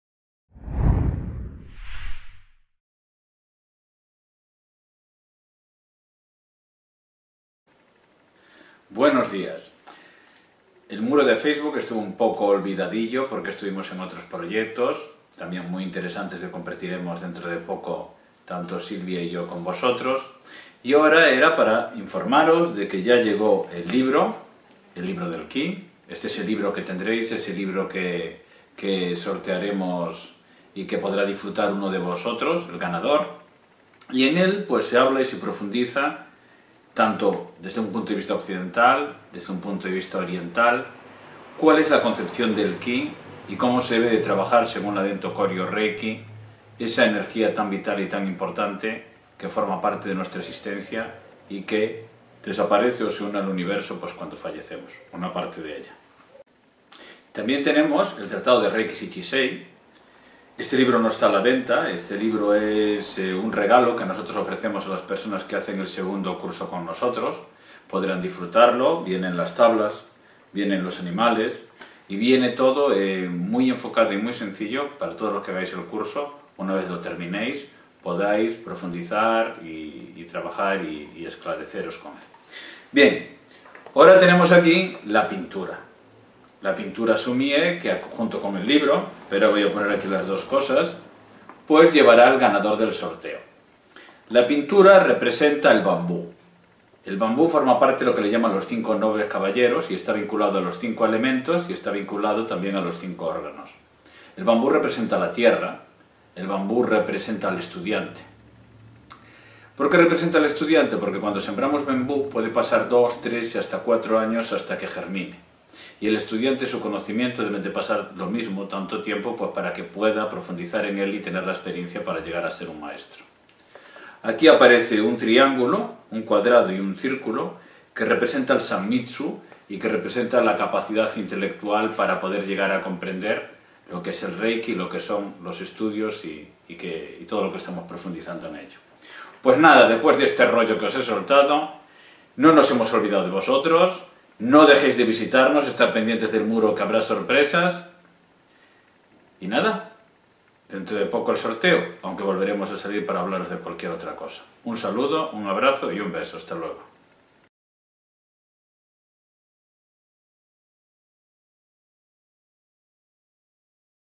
CAPITULO 22 YA LLEGO EL LIBRO
Ya llegaron los libros, tanto el de El REIKI, como el de Atrología Shichisei, y una breve explicación de la pintura SUMI E, que os regalamos.